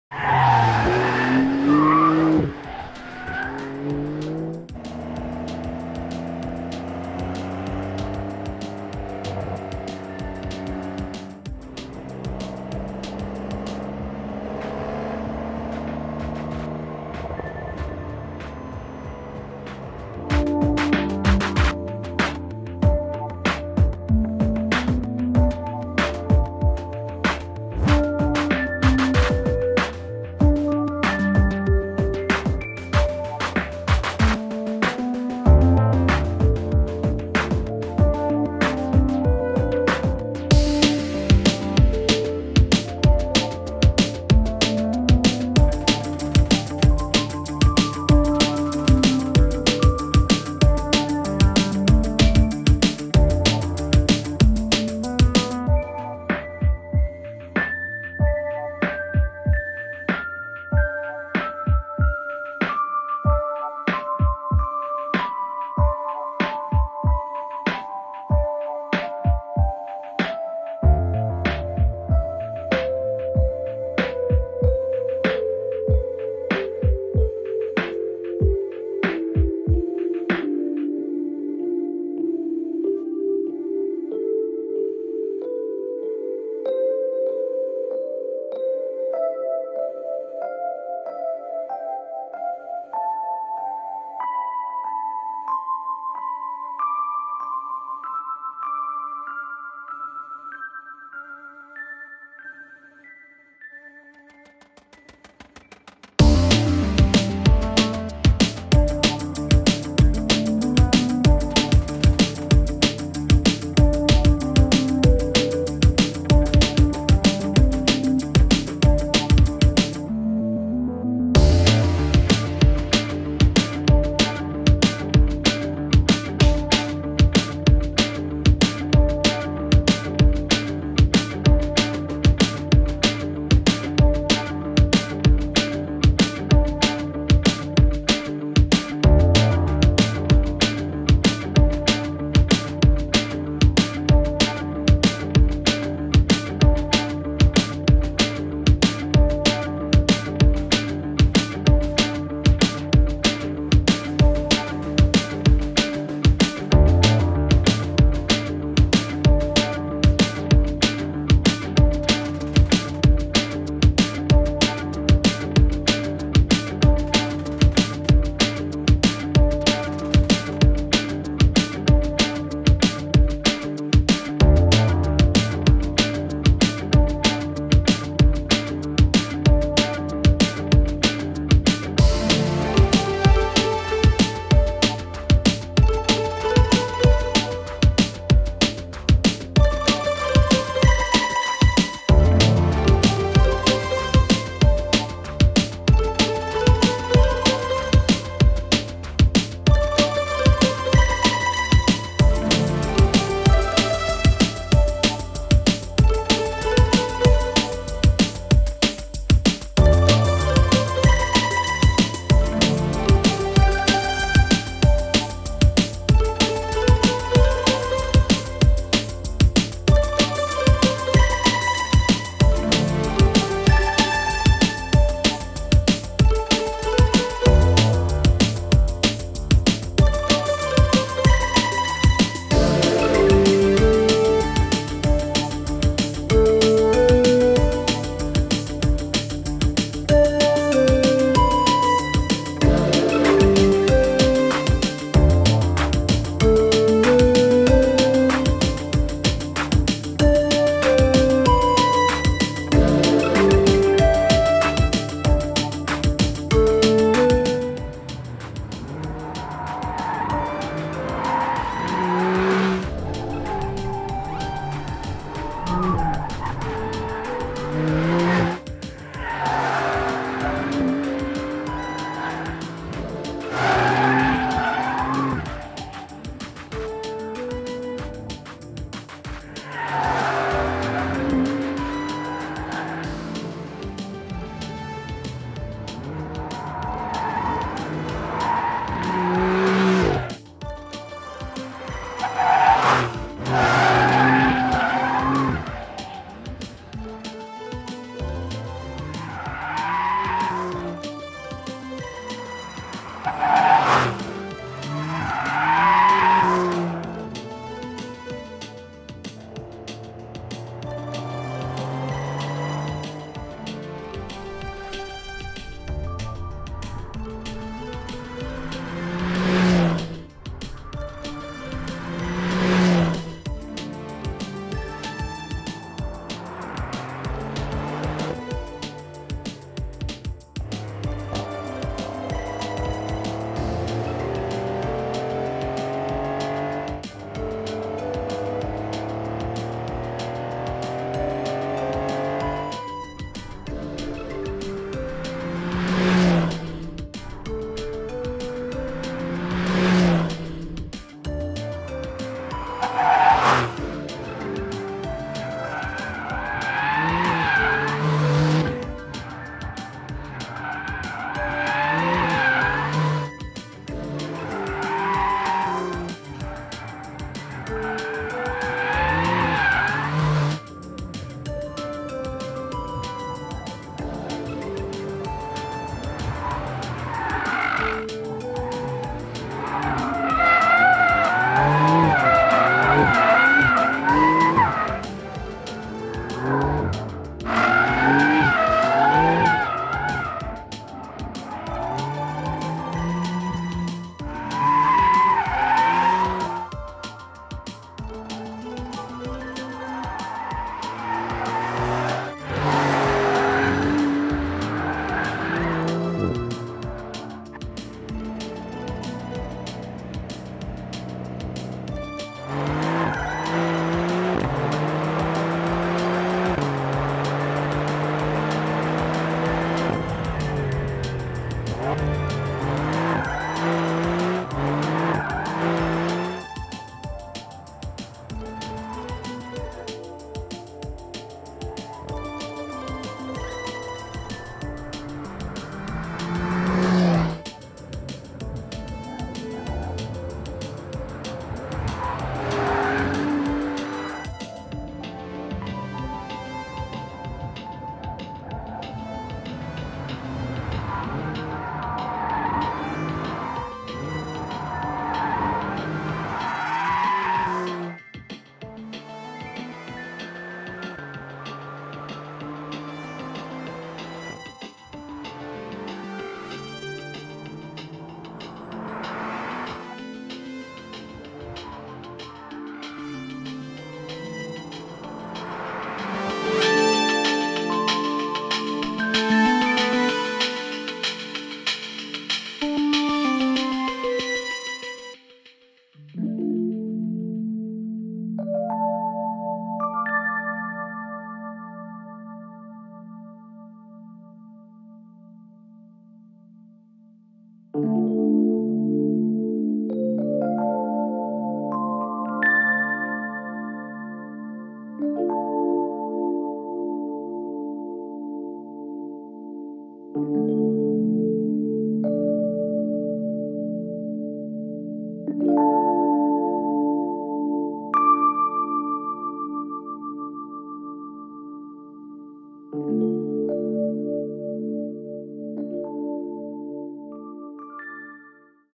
2022 PORSCHE 911 992 CARRERA GTS IN PYTHON GREEN | TRACK DRIVING + EXHAUST SOUND
DETAILS ABOUT THE CAR:
Review of the new Porsche 911 992 Carrera GTS in Python Green 2022 - crazy exhaust sound and driving, exterior and interior design.
Approximately two and a half years after the market launch of the 992, the 911 range is welcoming sporty GTS models to the family. Its six-cylinder boxer engine delivers 353 kW (480 PS).
Five model variants are now available at prices starting from 140,981 euros.
Porsche 911 Carrera GTS with rear-wheel drive, as Coupé and Cabriolet.
Porsche 911 Carrera 4 GTS with all-wheel drive, as Coupé and Cabriolet.
Porsche 911 Targa 4 GTS with all-wheel drive.